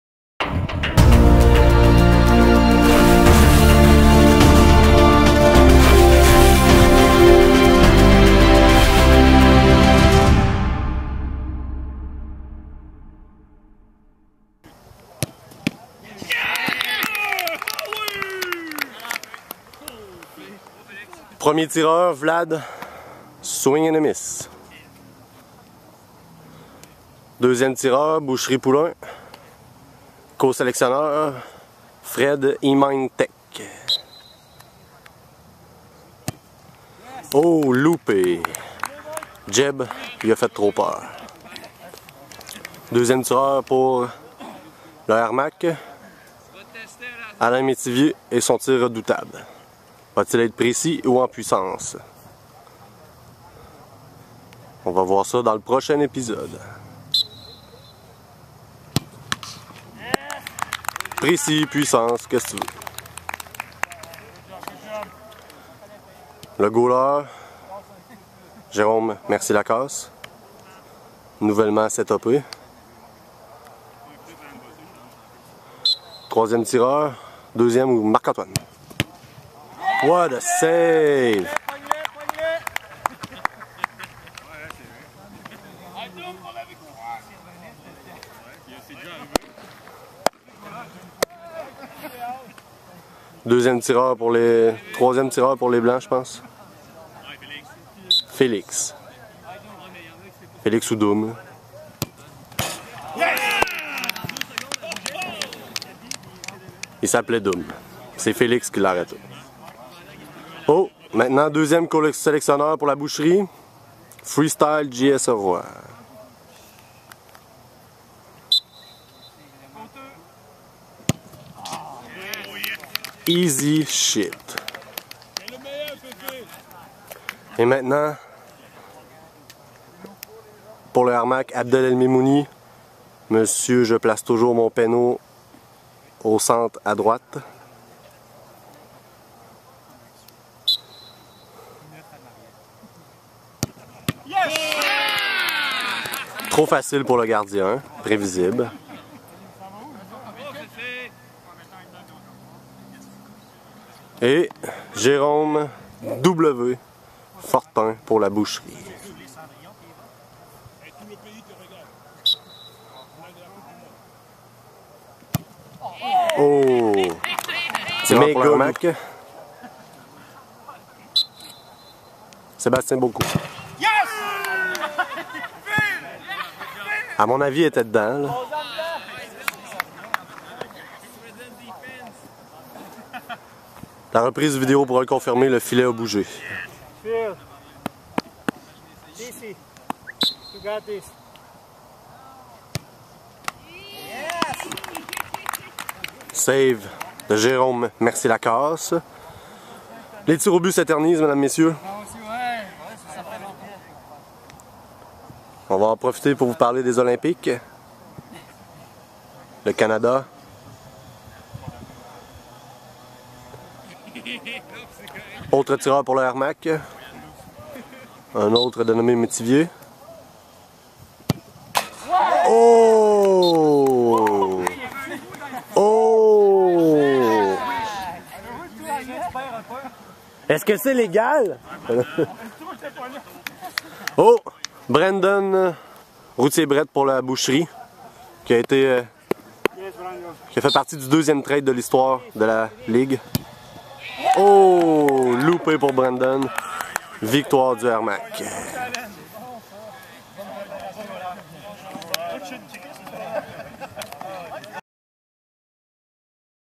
Semaine # 12, Tirs de barrage Boucherie Poulin vs Airmac
Intégrale des tirs de barrage entre la Boucherie Poulin et l'Airmac